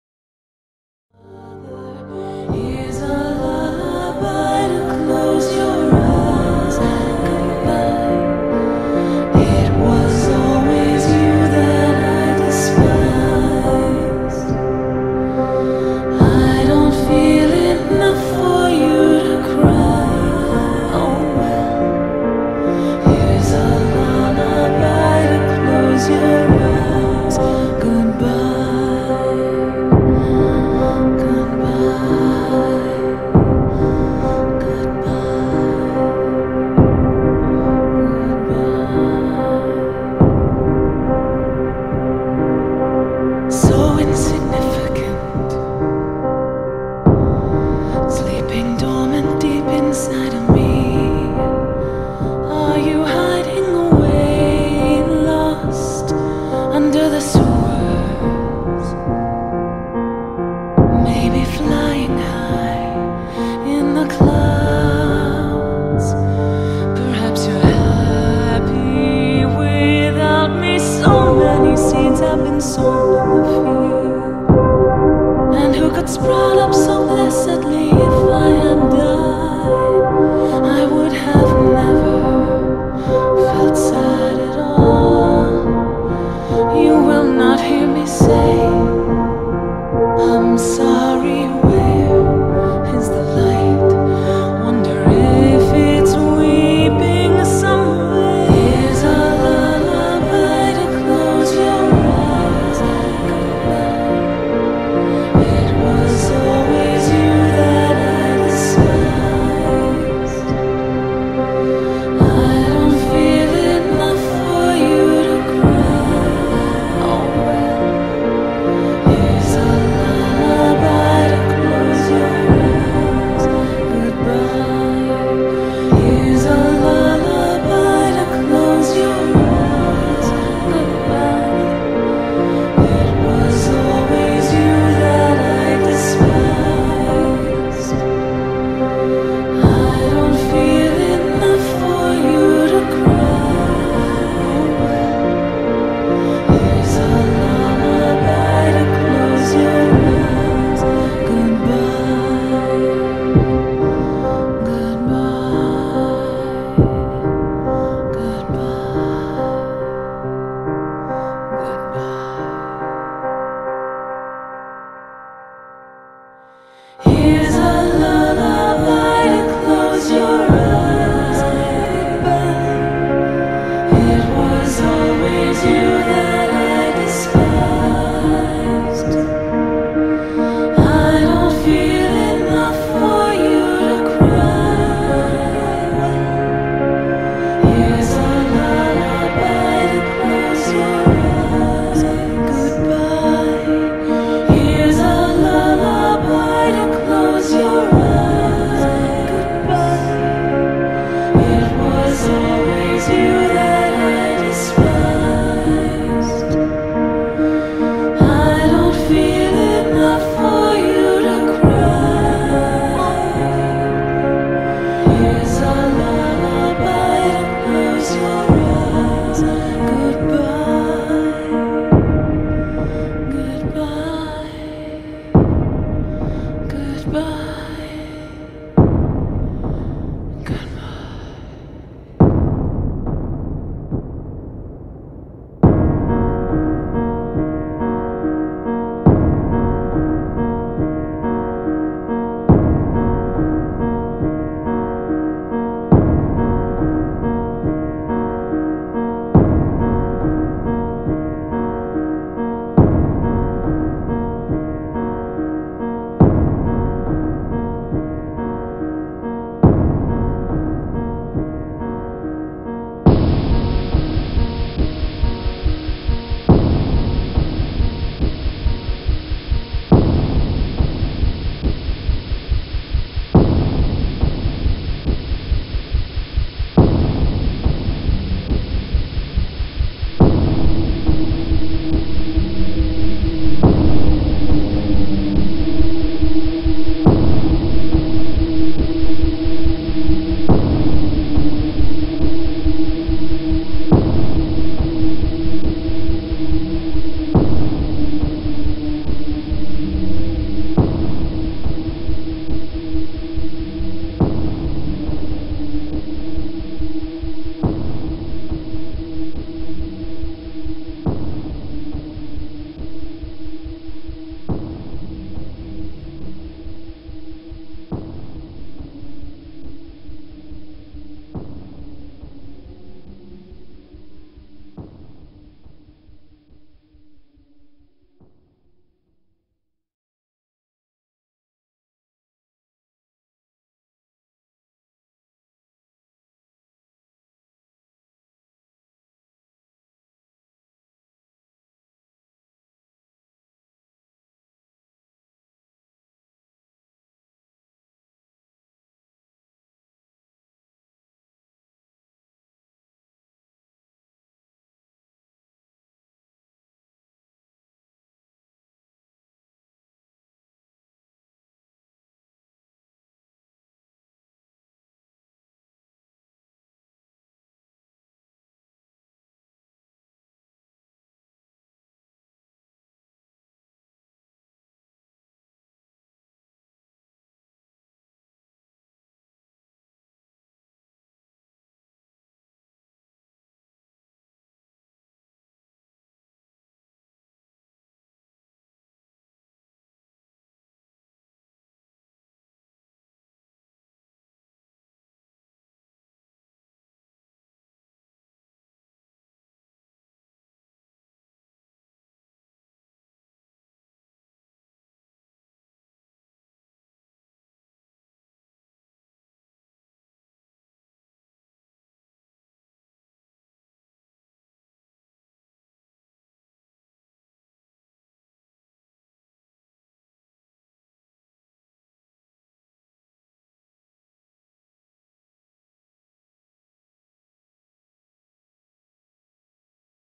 Karl Speedpaint
In celebration of my tablet's cord being found,I finally got off my butt and put my fanart of Karl from Law of Talos into digital
Layers-33
Program-Paint Tool Sai
Note:I hold no claims to owning the music played in this video,However I enjoy the song immensely, and believe it to fit this certain speedpaint.